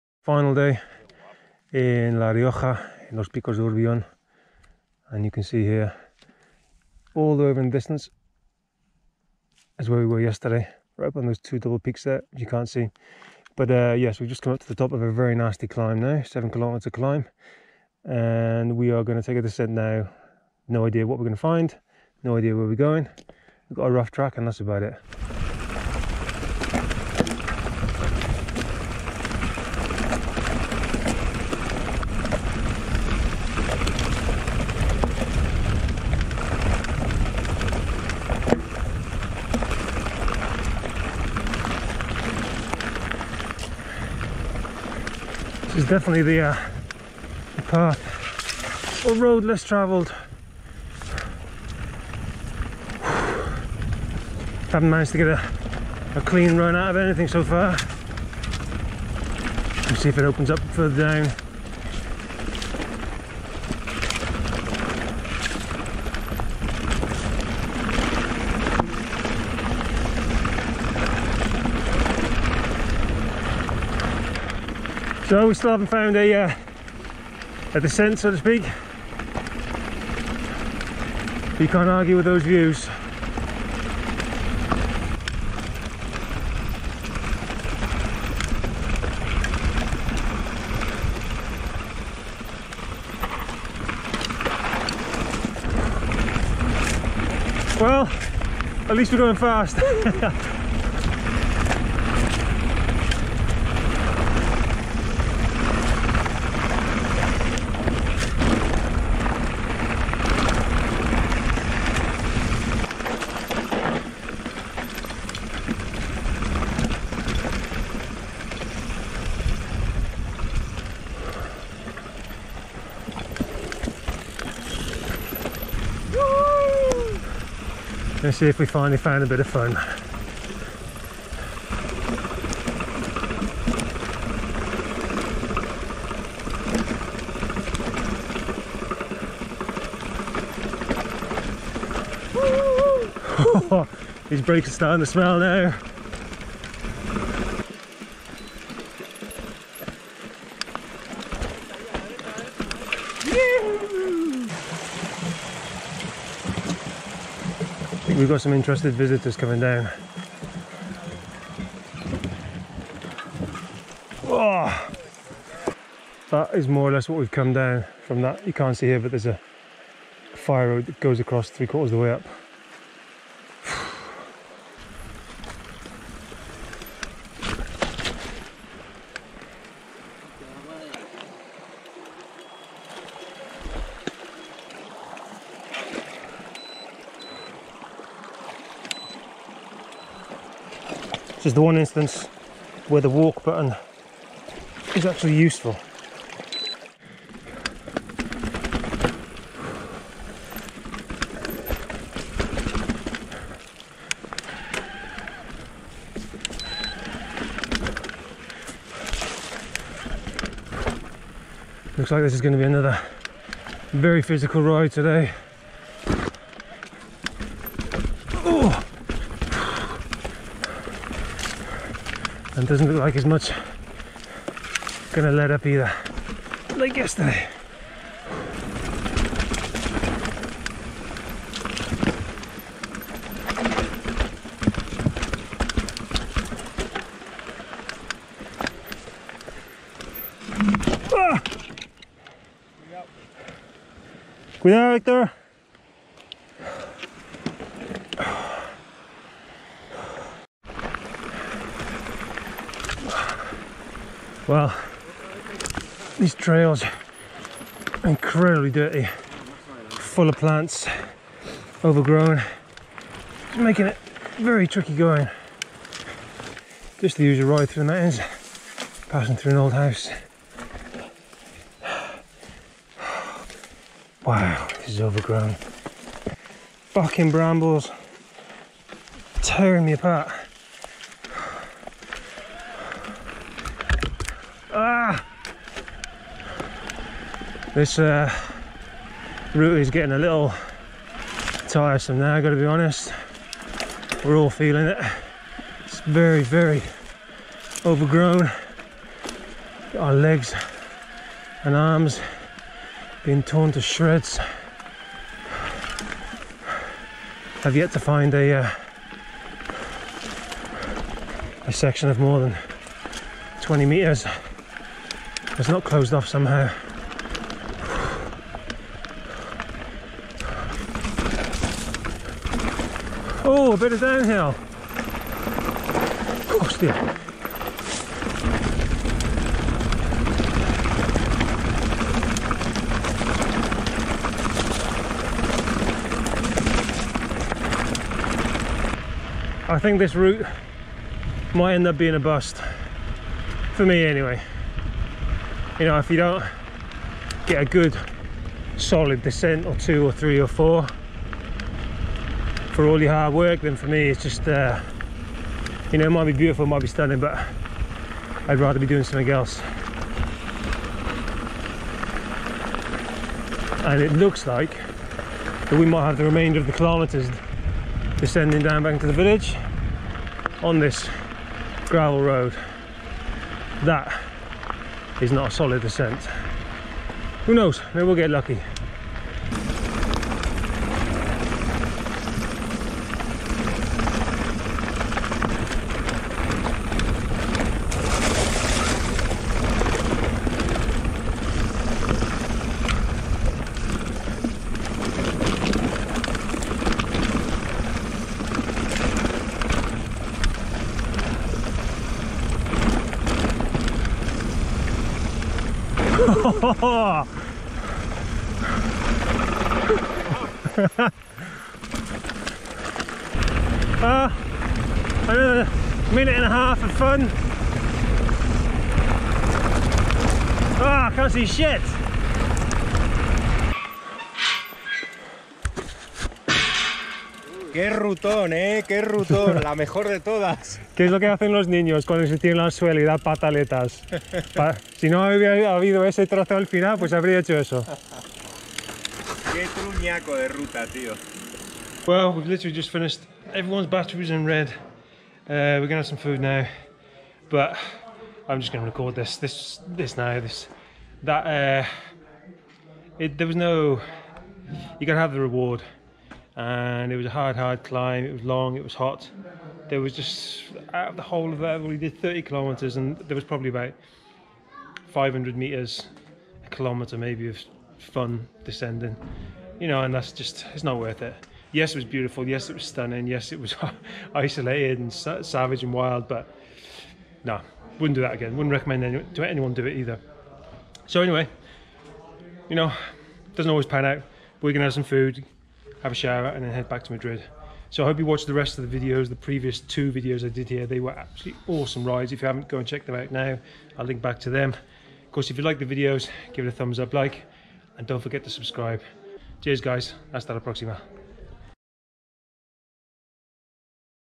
Final Day Of The Road Trip - Did We Go Out With A Banger?
This was the third and final day in Picos de Urbion, La Rioja. By this time I was a little tired after the previous three days riding. I was hoping for a banger to end the trip...

Today's ride was a complete unknown and had been promised by a local as being 100% clean and rideable.

Unfortunately, this ride was a complete bust. Closed off, a lot of firetrack and a bit of a downer to end, but you can't win them all.